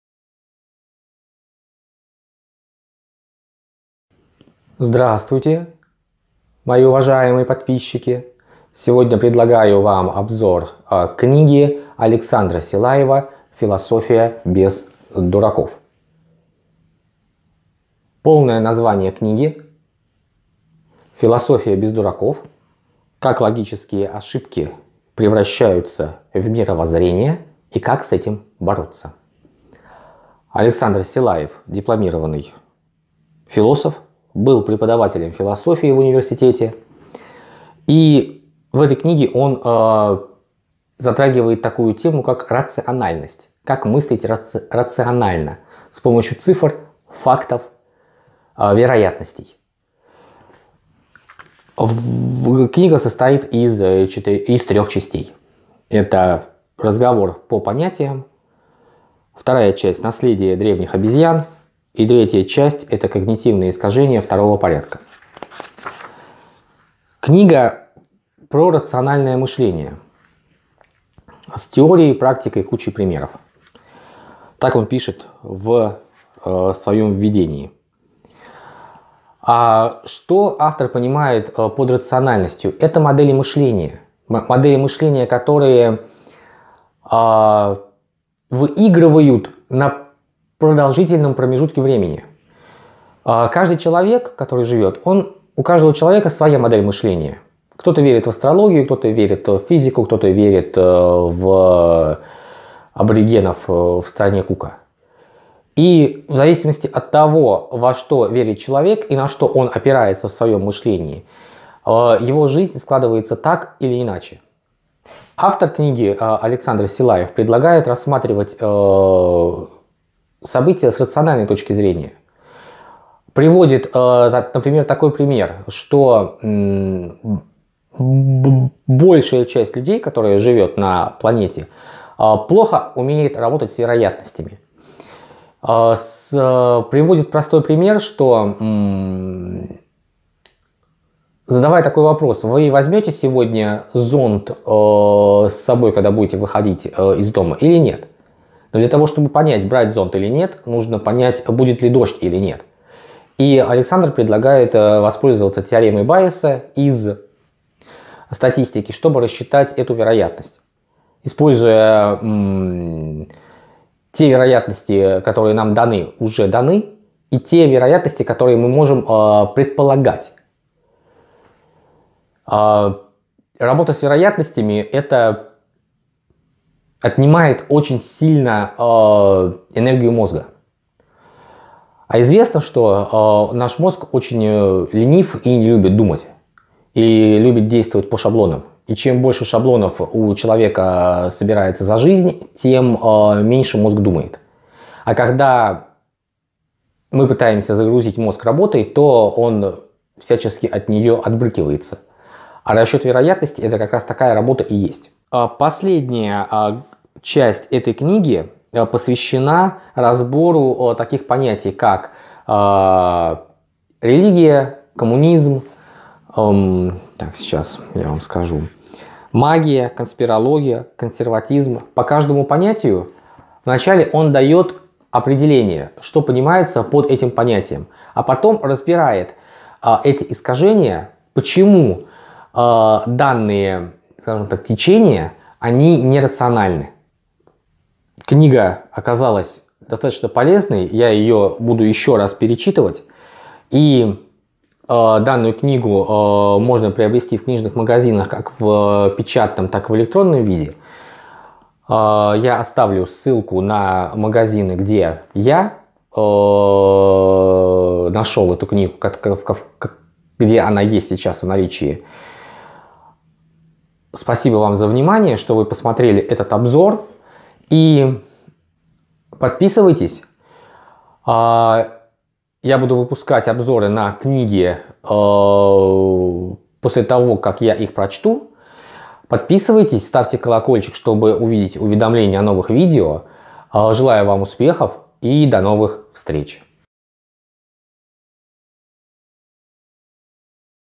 Александр Силаев - Философия без дураков. Обзор книги
Книга о том, как мыслить рационально.
Рационально - значит с использованием цифр, статистики, больших данных, не поддаваясь эмоциям и велению сердца.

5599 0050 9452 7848

Отзывы выходят на 2-4 недели раньше, чем на YouTube. Есть доступ к исходным материалам.

В видео представлен обзор на книгу Александра Силаева "Философия без дураков. Как логические ошибки становятся мировоззрением и как с этим бороться?".
Книга вышла в издательстве "Бомбора". Недостаток книги в том, что у неё нет ленточки-закладки (ляссе).

Коротко о книге.
Кому без рационального мышления жить хорошо? Тому, кто о нем даже не задумывается.
Автор призывает мыслить точно. Не путать чувства и мышление. Бороться с помощью конкретных методов с плохими теориями и логическими ошибками. Искать, допытываться, опровергать и сравнивать.
В этой книге про рациональное мышление и его врагов вас ждет не только теория, подкрепленная множеством примеров, но и практика, а также критика противников рационального мышления по основным направлениям.
Развивай рациональное мышление и борись с его врагами — главный девиз книги. Спойлеры — ошибки мышления повсюду. Хорошие новости — существуют конкретные методы борьбы с ними.
В своей книге автор:
- рассказывает о теориях и логических ошибках, которые рано или очень рано превращаются в мировоззрения;
- иллюстрирует теорию на конкретных примерах;
- предлагает методы и техники борьбы с противниками;
- учит мыслить точно, не путать чувства (даже аргументированные) с мышлением!

Постоянное увеличение критической массы идей и теорий, появляющихся каждый день, рискует отозваться ядерным взрывом в наших головах, заставляя тщательней поддерживать информационную гигиену и сохранять здравый смысл. Признайте, это не всегда удается, потому что проблема не персональная, а общечеловеческая. Автор предлагает прислониться к несущей стене в здании цивилизации - рациональности. Со всех сторон ее (рациональность и авторскую концепцию) поддерживают лучшие философские умы, чьи интеллектуальные достижения в книге критически переосмыслены (очищены от метафизической пыли) и умело конвертированы в практические упражнения, понятные советы и яркие примеры.